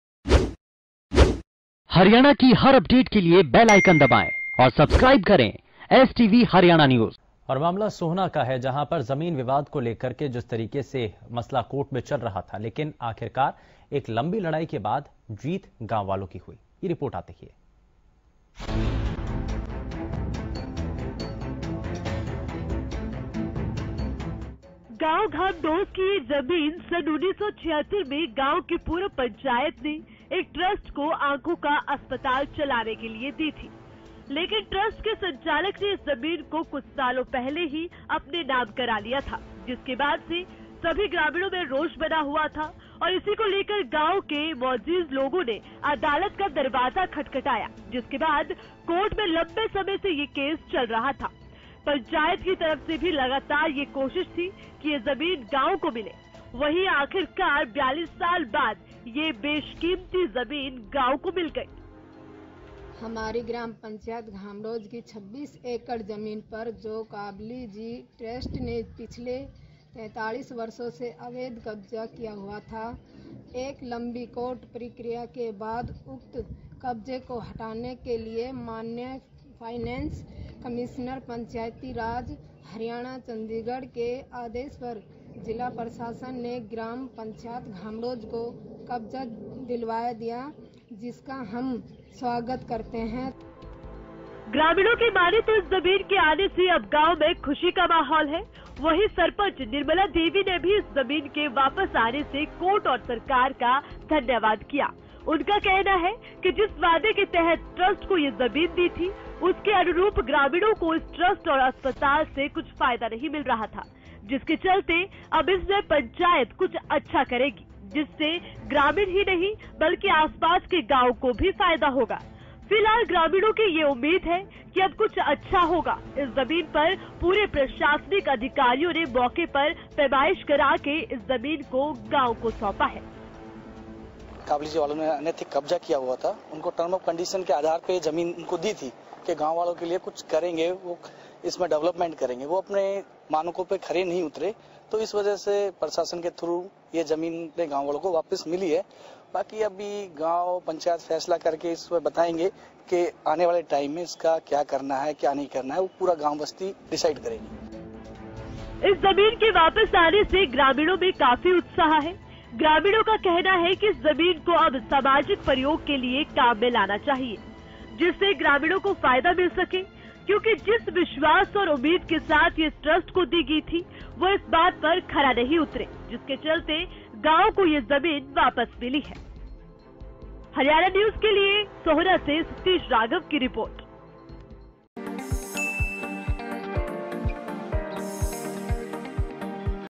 42 साल की अदालती लड़ाई के बाद वापस मिली जमीन, कोर्ट के आदेश के बाद गांव ने लिया कब्जा
सोहना के गांव घामडौज को 42 साल तक अदालती लड़ाई लड़ने के बाद आखिर गांव की जमीन वापस मिल ही गई.  बता दे कि गांव ने ट्रस्ट के खिलाफ कोर्ट में केस किया हुआ था.  कोर्ट ने तमाम दलील सुनने के बाद गांव के पक्ष में ये फैसला सुनाया और गांव को साढ़े 26 एकड़ जमीन वापस मिल गई है.